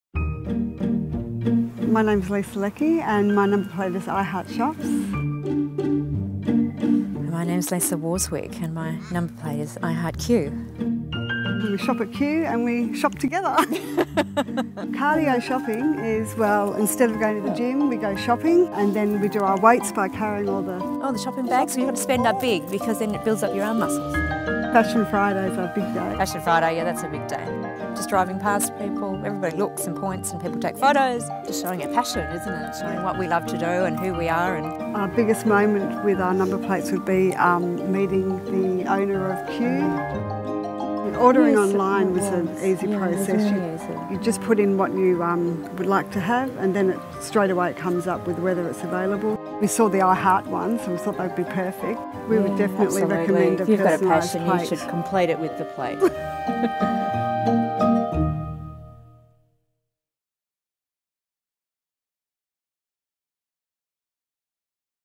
Custom Plates:IHeartSHOPS
Transcript:

WOMAN 1: My name is Lisa Leeky and my number plate is I Heart Shops.

WOMAN 2: My name's Lisa Warswick and my numberplate is I Heart Cue.

WOMAN 1:We shop at Cue and we shop together. (laughs)

WOMAN 1: Cardio shopping is well, instead of going to the gym we go shopping and then we do our weights by carrying all the...  shopping

WOMAN 2: ...all the shopping bags you've got to spend up big cause then it builds up your arm muscles.

WOMAN 1: Fashion Fridays our big day.

WOMAN 2: Fashion Friday yeah that's a big day.

WOMAN 1: Just driving past people, everybody looks and points and people take photos.

WOMAN 2: Just showing our passion isn't it and showing what we love to do and who we are.

WOMAN 1: Our biggest moment with our number plates would be um meeting the owner of Cue.

WOMAN 1: Ordering online was an easy process you ..

WOMAN 2: ...yeah it was really easy...

WOMAN 1:  ... You just put in what you'd like to have and then it straight away comes up with whether it's available.

WOMAN 1: We saw the I heart ones and we thought they'd be perfect.

WOMAN 1: We would definitely recommend a personalized plate.

WOMAN 2: Yeah absolutely if you've got a passion you should complete it with the plates.